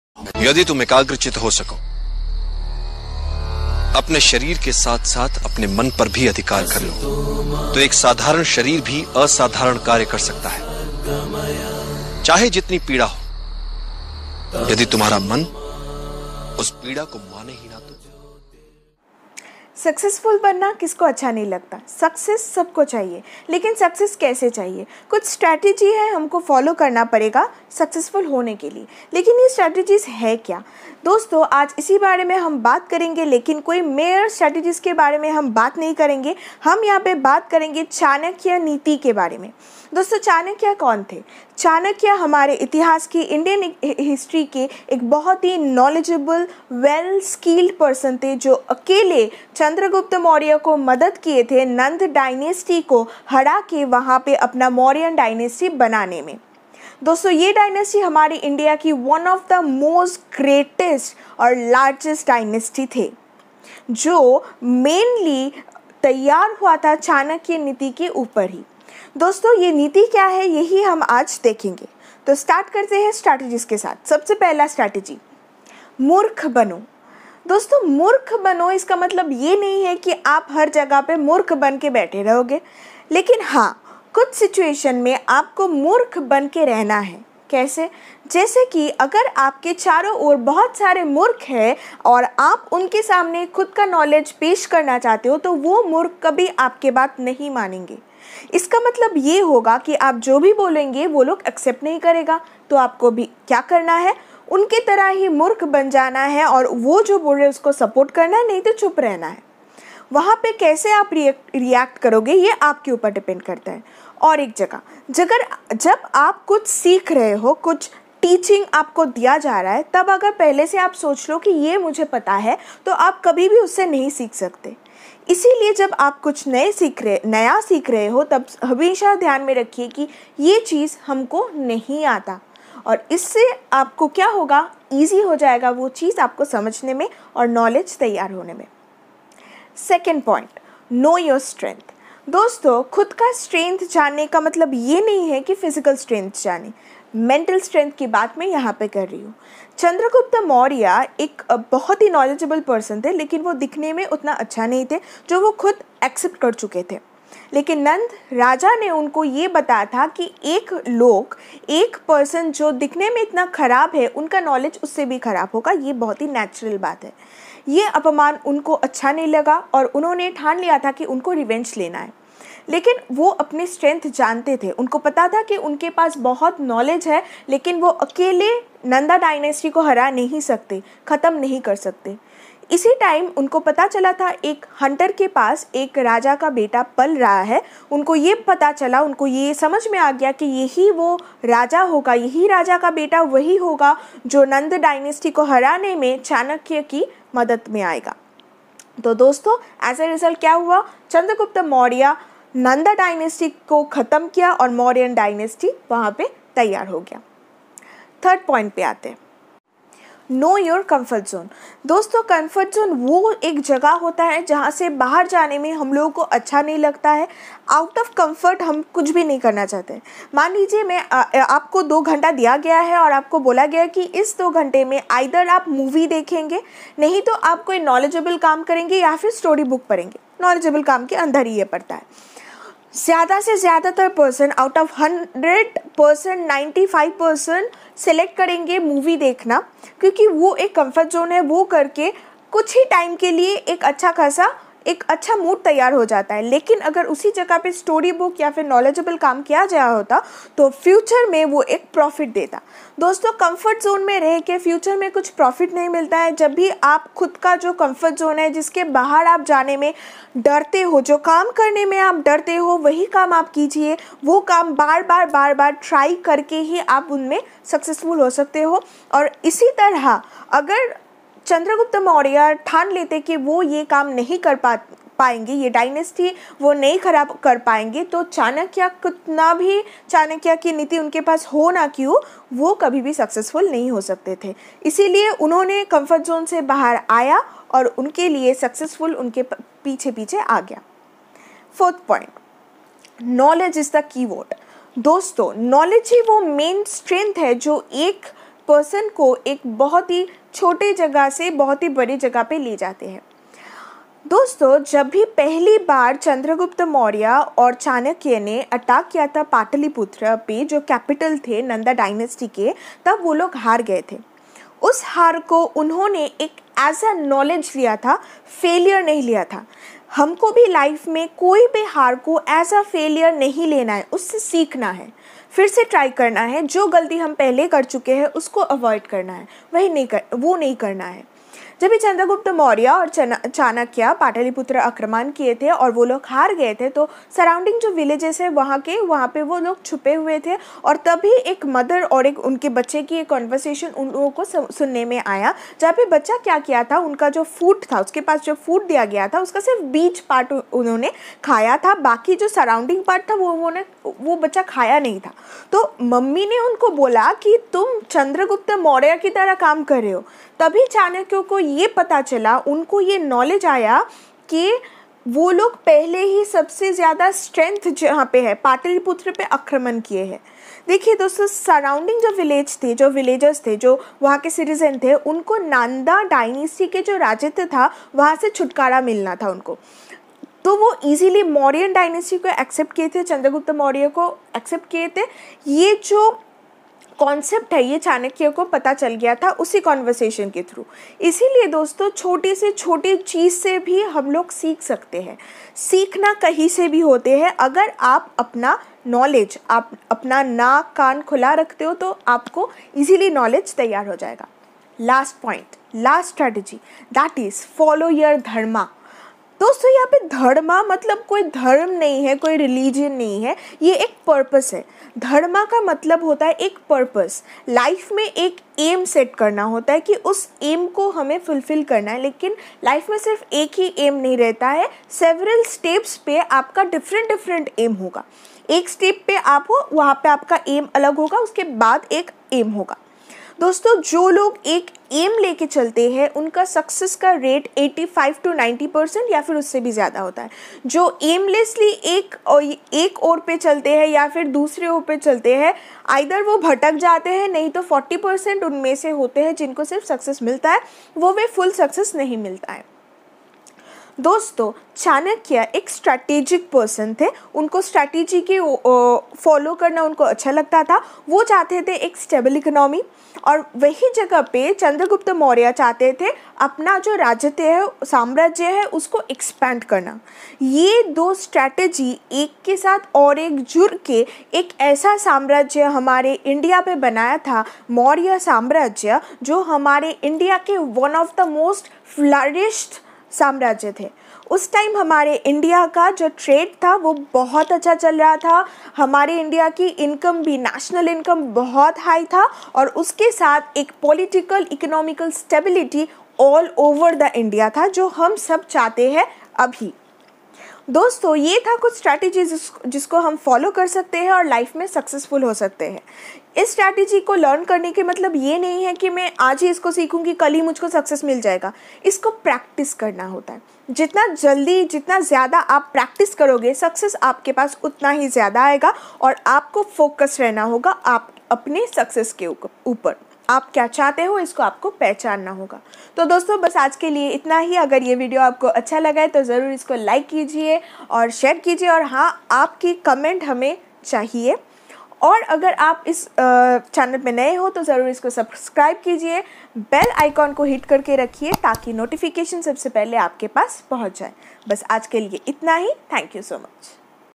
5 Best Chanakya niti to become successful in life..Chanakya nitis.
Hello viewers,
Success needs planning and strategic movement. But what strategies are to be followed is the big question.
Here, in this video we have shared the strategies of Chanakya to become successful.
Thank you for you continuous love and support.